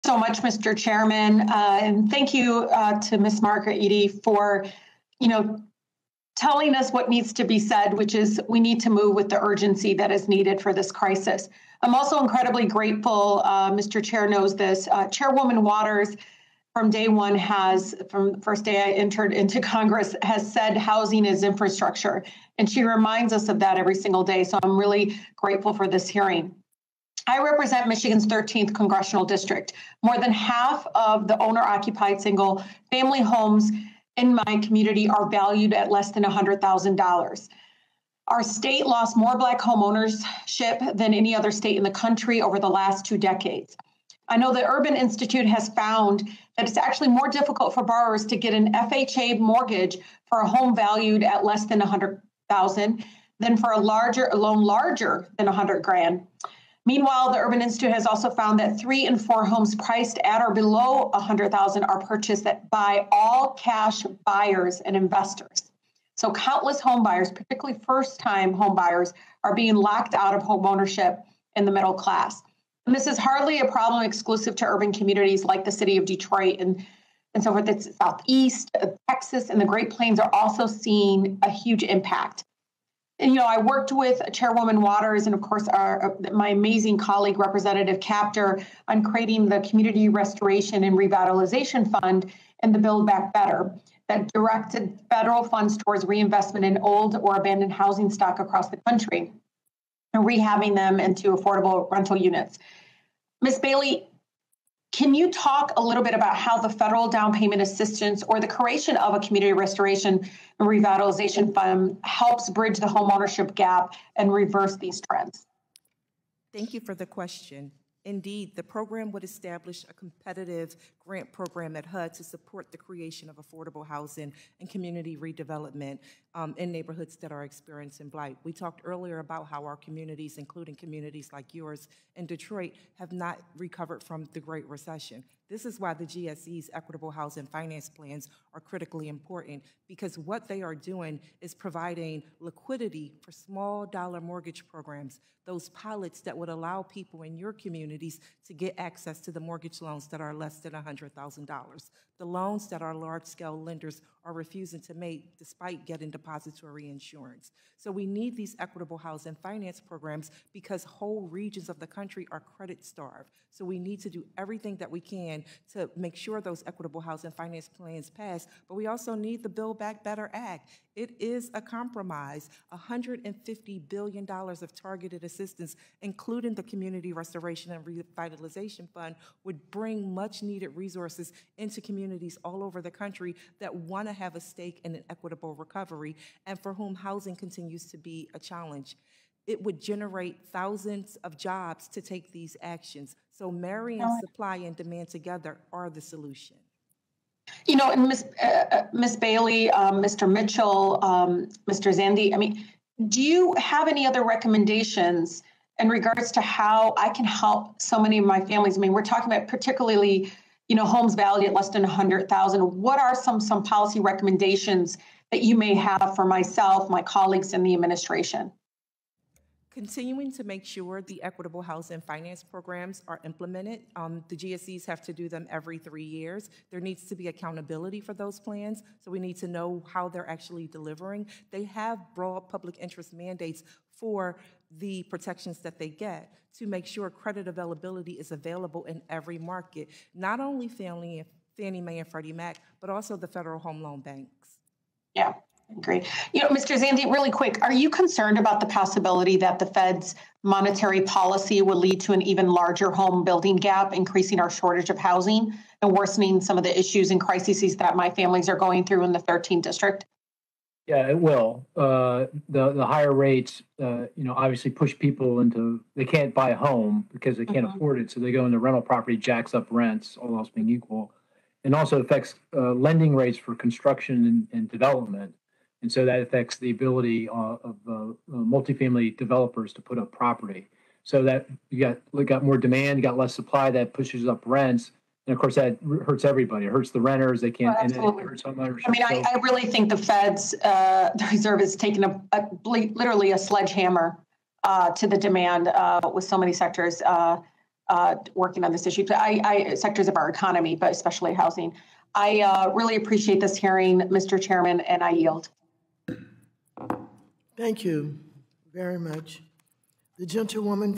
Rashida Tlaib: ’First-Time Home Buyers Are Being Locked Out’ Due To Housing Prices
During the House Financial Services Committee hearing on Thursday, Rep. Rashida Tlaib (D-MI) questioned witnesses on loan practices and the disadvantages the lower and middle classes face in owning a home.